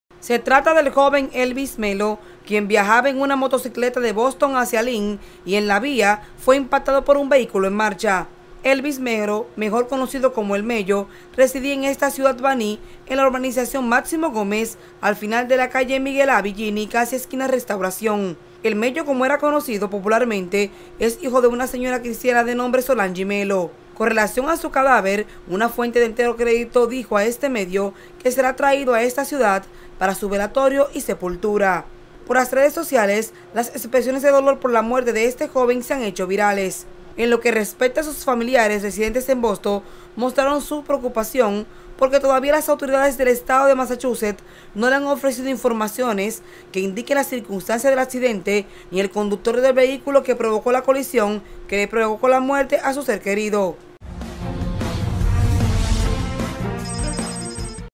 MUERE EN ACCIDENTE BANILEJO EN LYNN
Consternación ha causado en esta ciudad Bani la muerte de forma accidental en Lynn, de un joven que gozaba de mucho aprecio por su buena formación familiar y por su dedicación al trabajo.